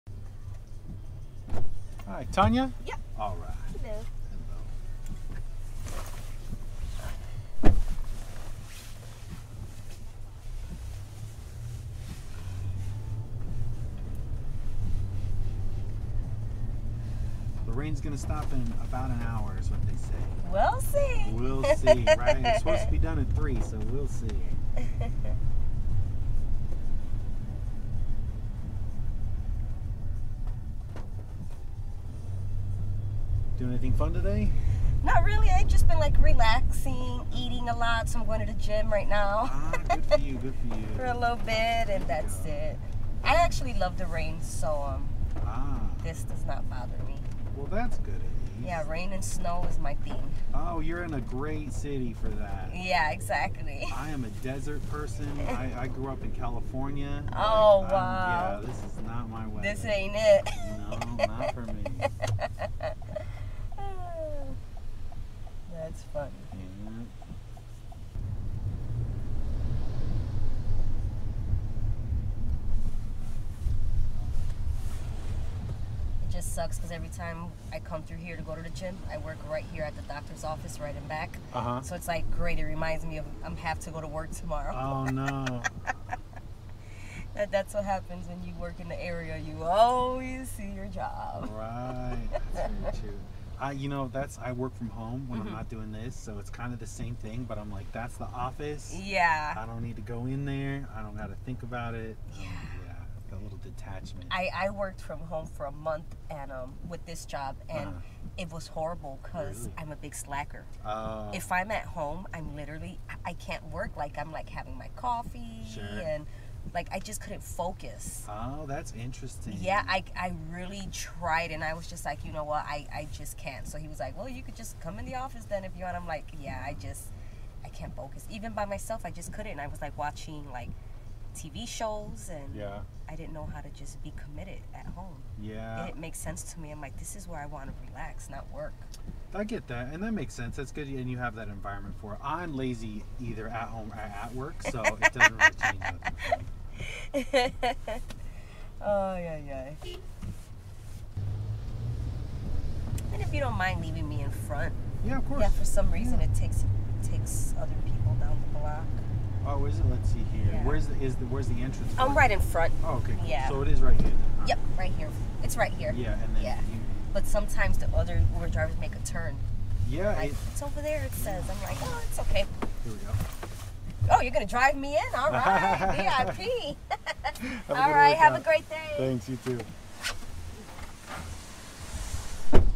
EP71: Tanya - Working from Home
This was a fun short ride. We talked about working from home and the pluses and minuses.